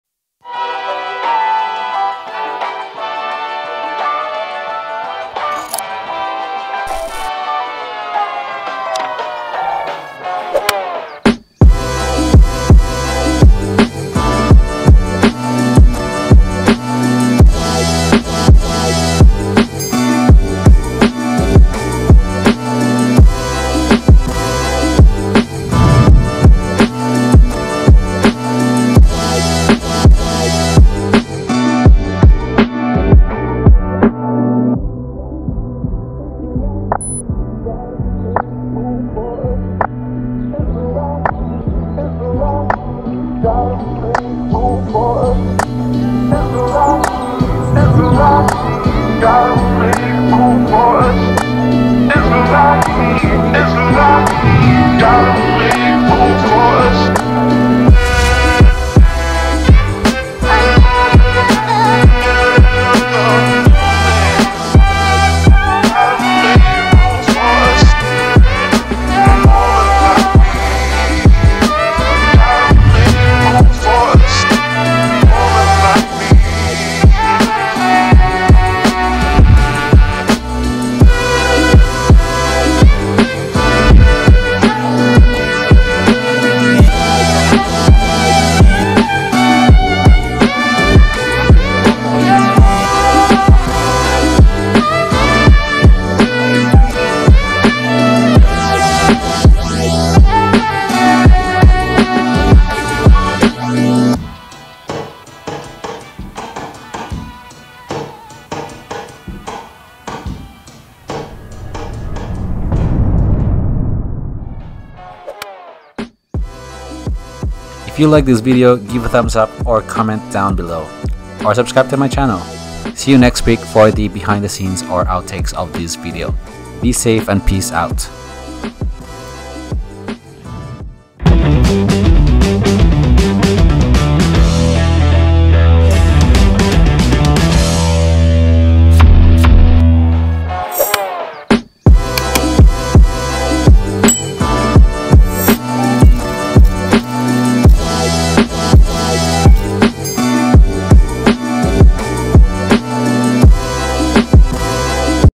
Salon Cinematic B Roll with Sony A73
Salon Cinematic B Roll with Sony A7III 20mm

0:00 Opening
1:56 Outtro

Few weeks ago, I had the opportunity to be invited to the opening of the salon, MJ Skinfinity, in Chiba Prefecture to take a video and make a promotional-type of video. Without any experience with this type of video, I just grabbed my Sony A7III and DJI Ronin to take the challenge. And this is the outcome.
Note:20mm 1.8f lens was used throughout the whole shoot.

Warning!!!

📷Sony a7iii
🎬Video Editor : DaVinci Resolve -

Warning!!!